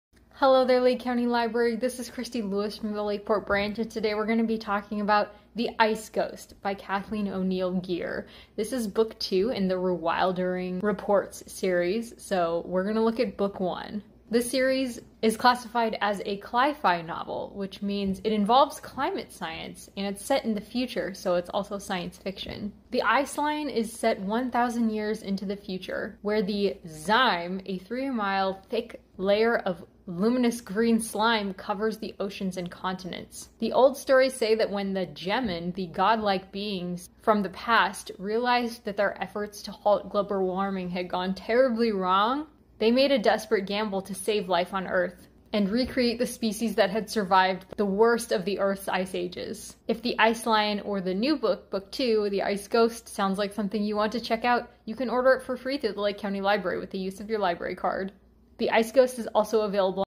The Ice Ghost by Kathleen O'Neal Gear
The Ice Ghost by Kathleen O'Neal Gear is a new science fiction novel